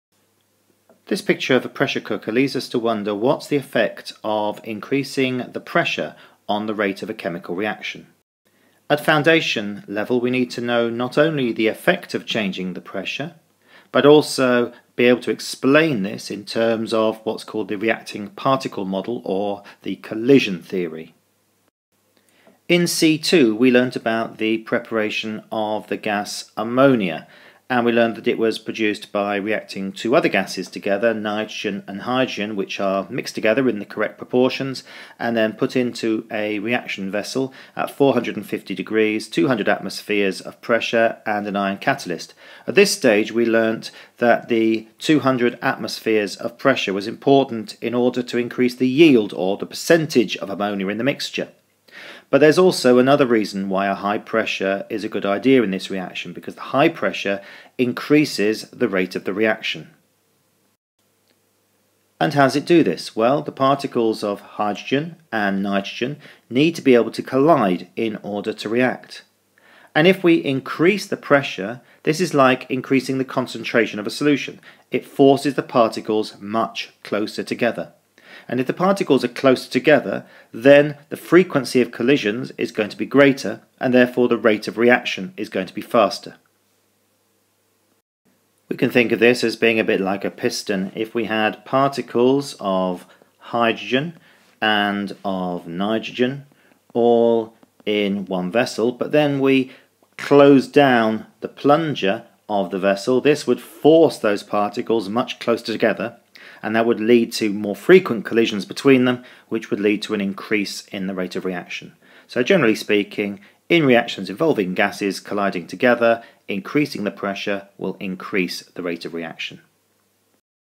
OCR Gateway B C3 Reaction Rates - Pressure (Foundation)
Tutorial which looks at the effect of an increase in pressure on reactions involving gases, and explains this using the reacting particle model.